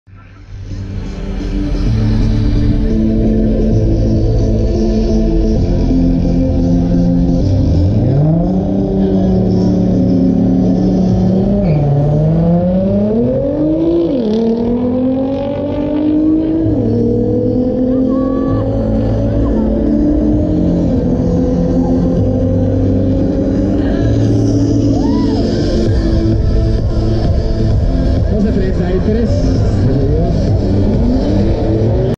Nissan GT-R 2017 vs Porsche 911 Carrera | Octubre 2017
Arrancones 1/4 de Milla Centro Dinámico Pegaso Toluca, México Octubre 2017